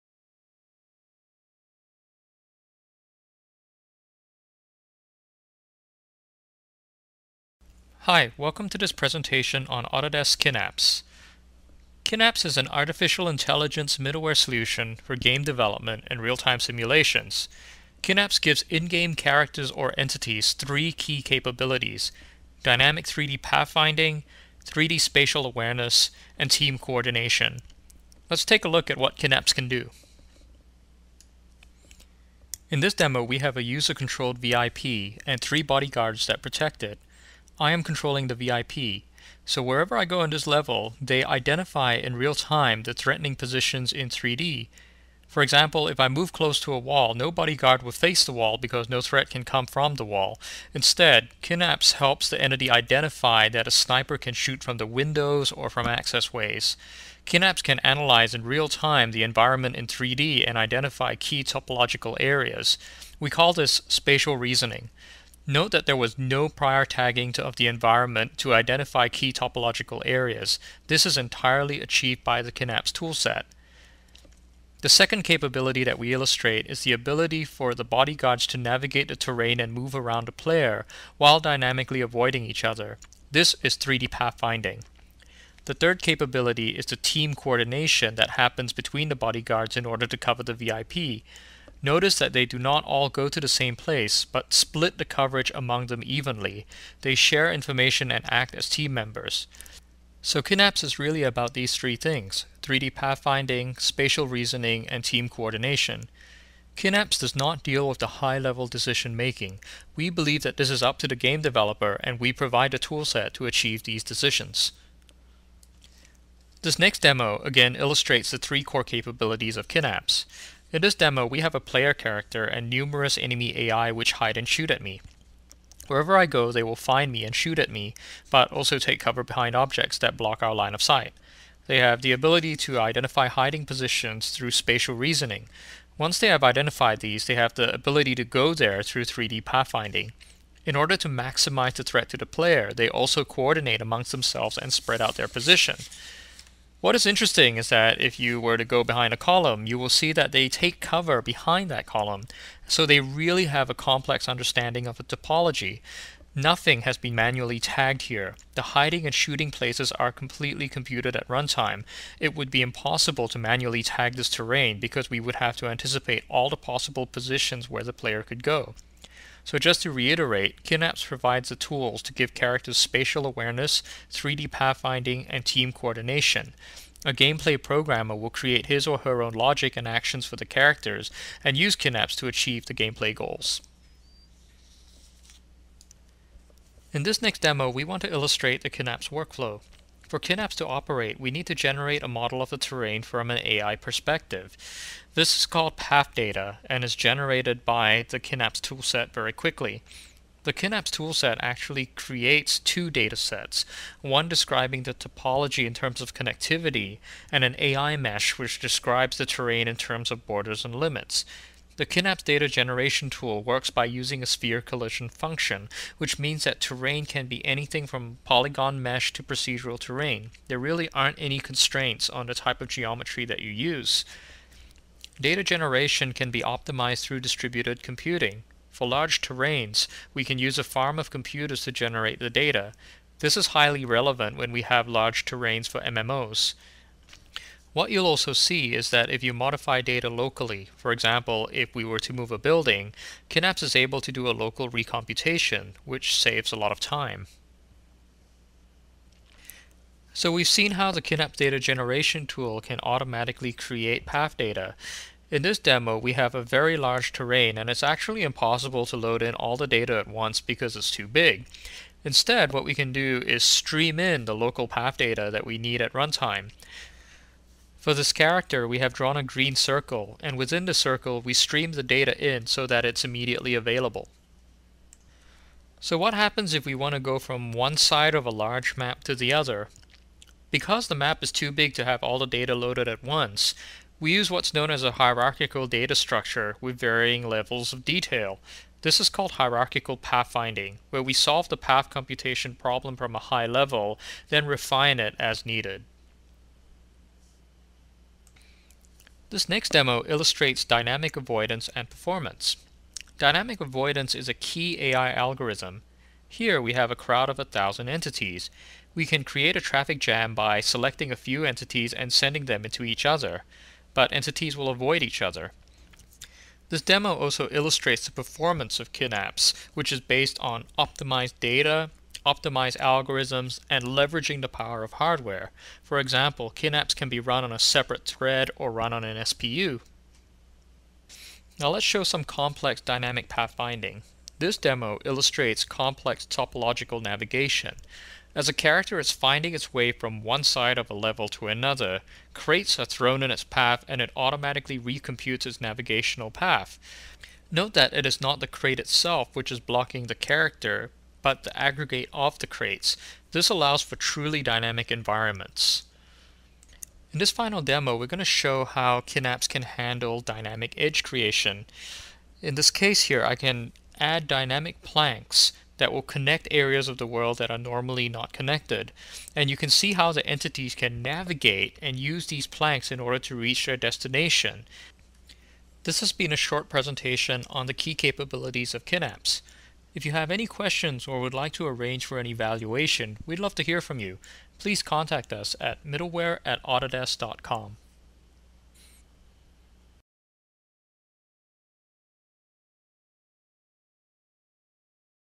Autodesk Kynapse Demonstration
This game development software video provides several demos of Autodesk® Kynapse® features such as dynamic avoidance, 3D pathfinding, spatial awareness, and team coordination.

Autodesk® Kynapse® is an artificial intelligence middleware that helps game developers build more compelling character behaviors in video games. It offers high performance pathfinding within dynamic environments, and automated tools for generating AI data. Kynapse has been successfully integrated into at least 100 high profile games of different genres. Using Kynapse, developers can help improve game play while reducing development costs and cutting time to market.

The Kynapse runtime is a high performance C++ library for handling real-time AI calculations. Using the Kynapse C++ API, developers can create more complex, more compelling behaviors for in-game characters.